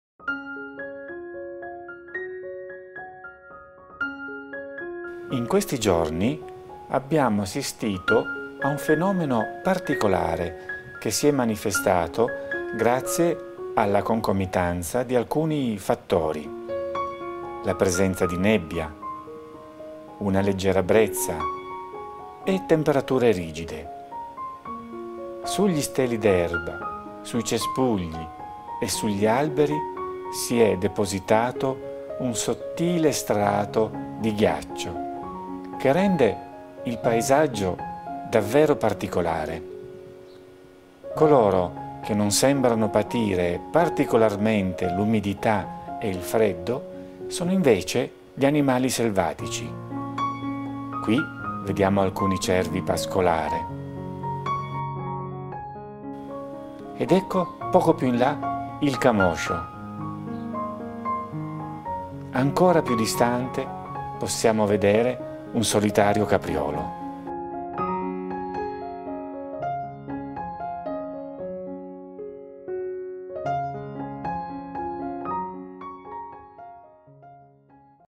Il paesaggio imbiancato - I Parchi delle Alpi Cozie – Piccoli frammenti di natura e cultura
In questi giorni abbiamo assistito a un fenomeno particolare che si è manifestato grazie alla concomitanza di alcuni fattori: la presenza di nebbia, una leggera brezza e temperature rigide.
Sugli steli d’erba, sui cespugli e sugli alberi si è depositato un sottile strato di ghiaccio che rende il paesaggio davvero particolare. Coloro che non sembrano patire particolarmente l’umidità e il freddo sono invece gli animali selvatici. Qui vediamo alcuni cervi pascolare. Ed ecco il camoscio. Poco più in là un solitario capriolo.

Per saperne di più vi invitiamo a visitare il nostro sito